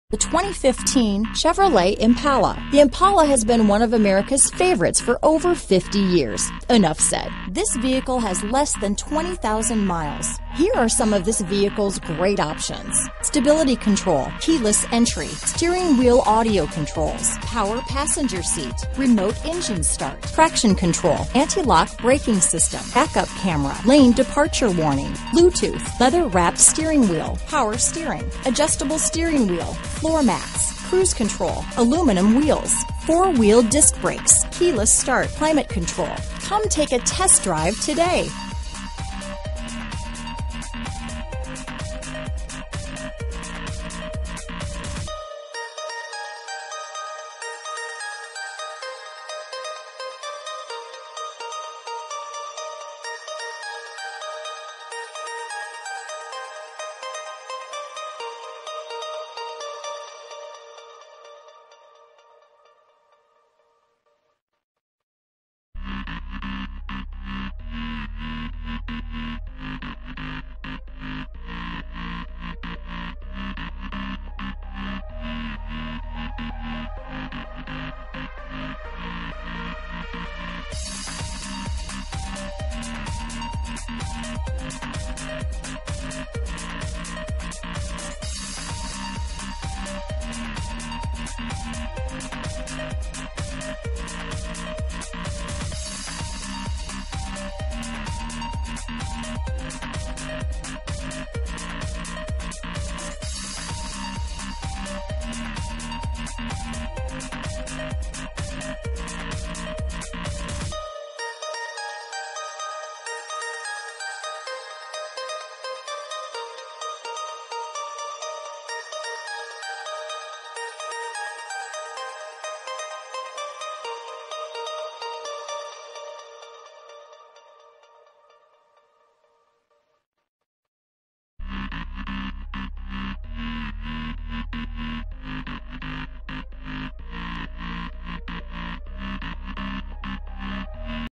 2015 Chevrolet Impala Phoenix, Glendale, Peoria, Sun City, Surprise, AZ 490293
Iridescent Pearl Tricoat Used 2015 Chevrolet Impala available in Phoenix, Arizona at Bell Road Toyota. Servicing the Glendale, Peoria, Sun City, and Surprise, AZ area.

Check out this 2015 Chevrolet Impala LTZ. Its Automatic transmission and Gas/Ethanol V6 3.6L/217 engine will keep you going.